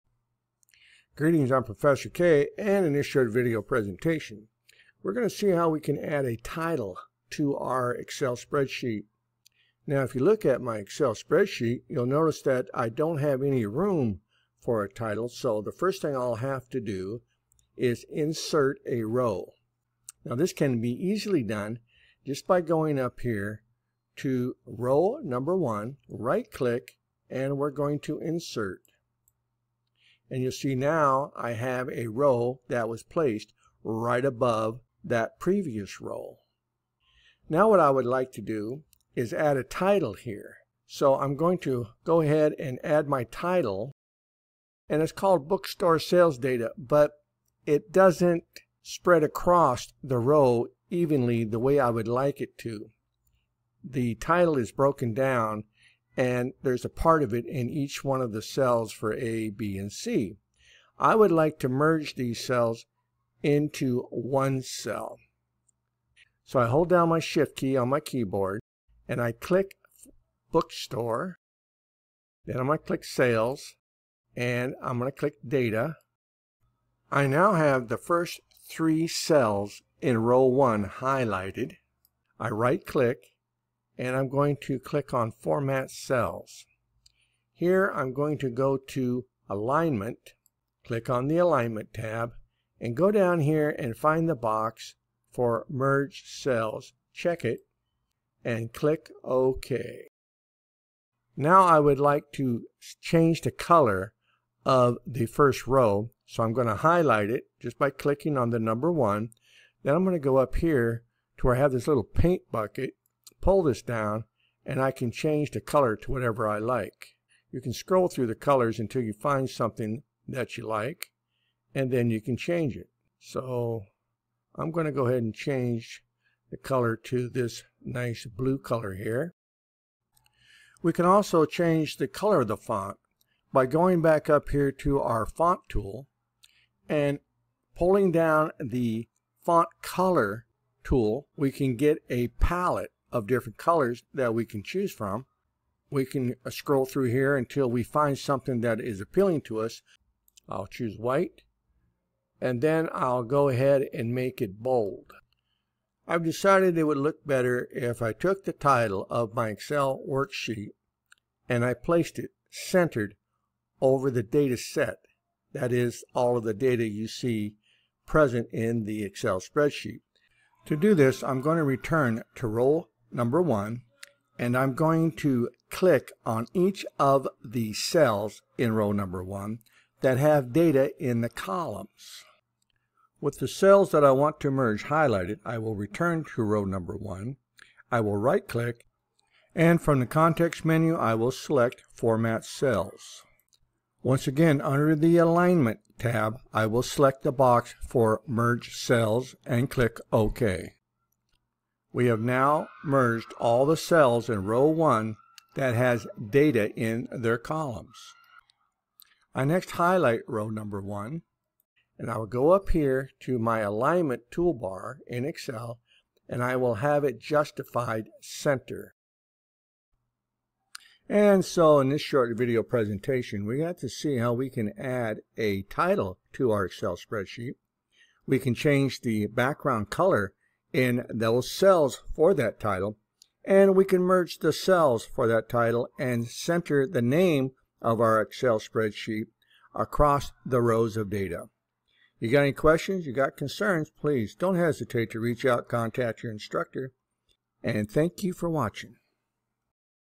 Adding a Title to an Excel Spreadsheet
In this quick Excel tutorial, you'll learn how to add a clear, professional title to your spreadsheet using just a few simple steps. Whether you're organizing data for school, work, or personal use, a well-placed title makes your document easier to read and understand.

✅ What You’ll Learn:

Where to place your title for best visibility

How to merge and center cells for a clean look

Tips for formatting your title (font, size, color)

Making your spreadsheet presentation-ready

🖥️ Perfect for beginners and anyone wanting to improve their Excel skills!